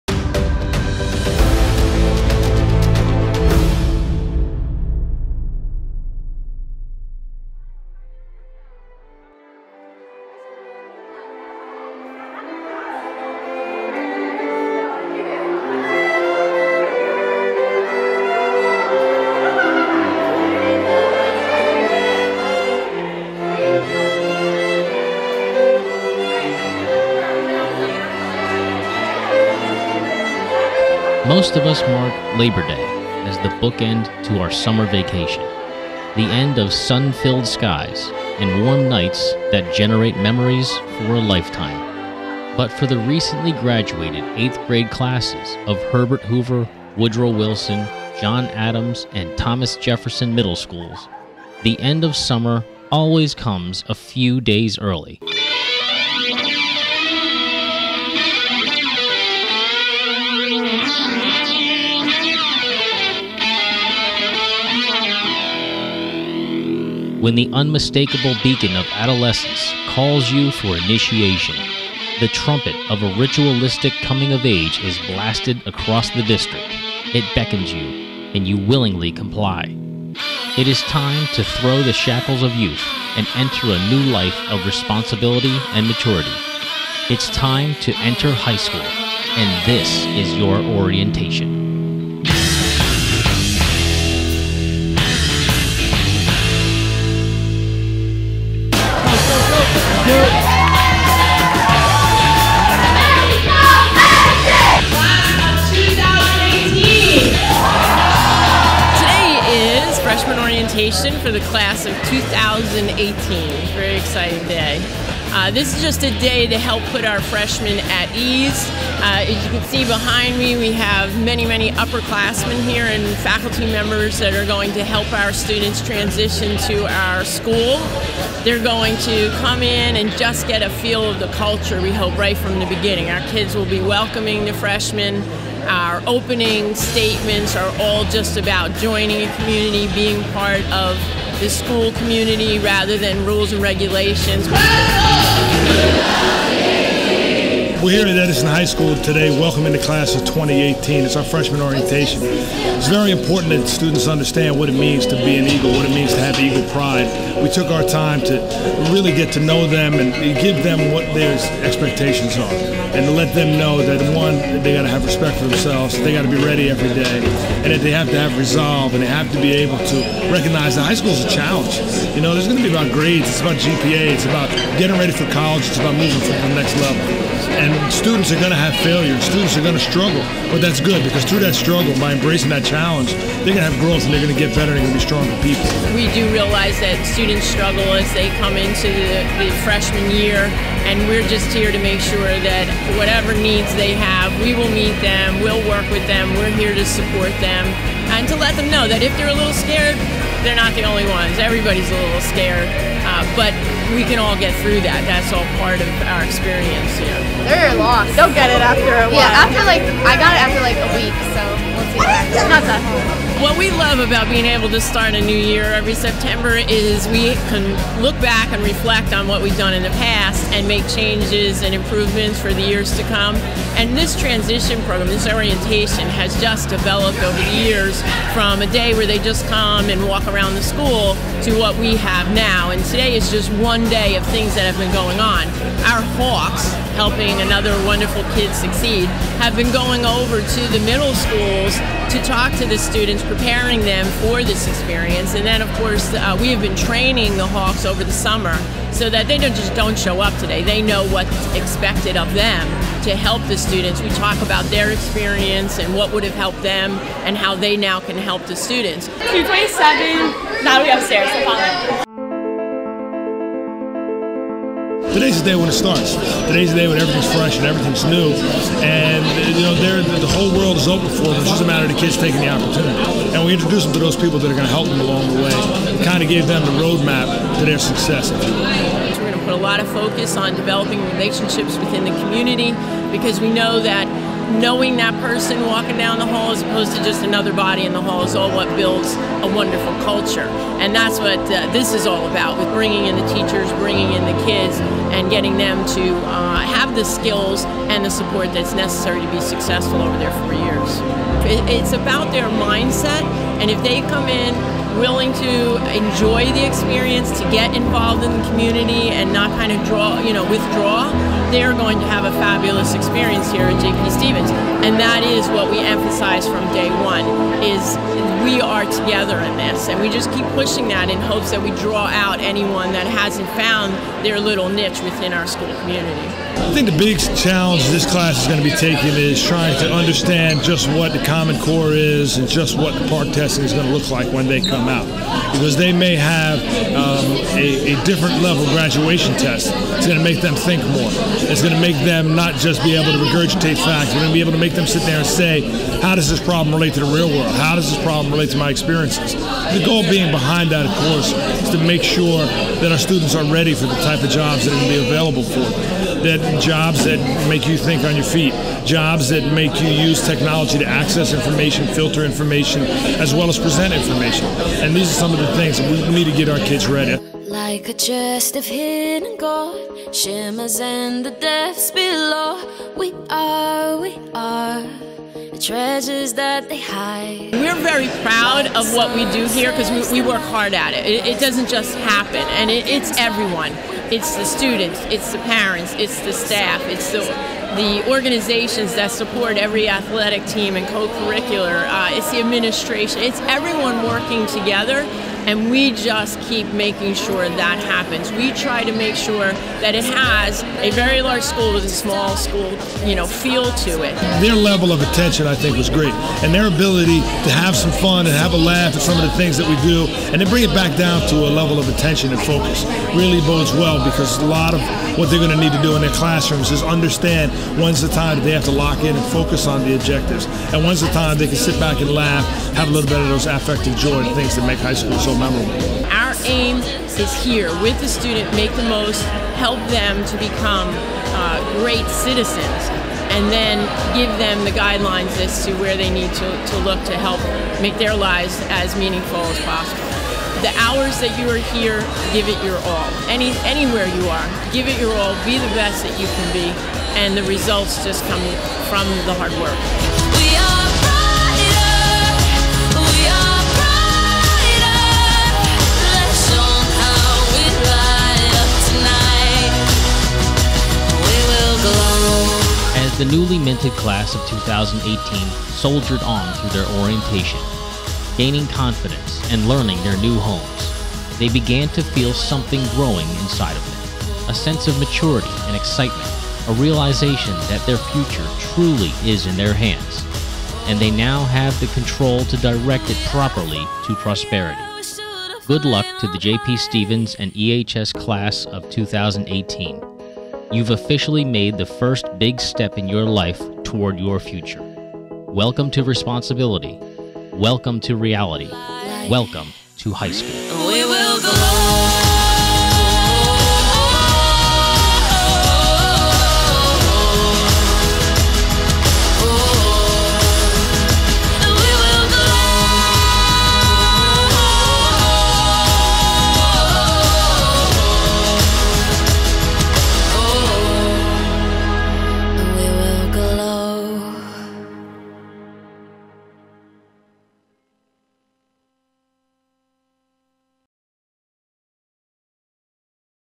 Freshman Orientation Day 2014 for Edison High School and JP Stevens
Freshman Orientation Day 2014 for Edison High School and JP Stevens, in Edison New Jersey.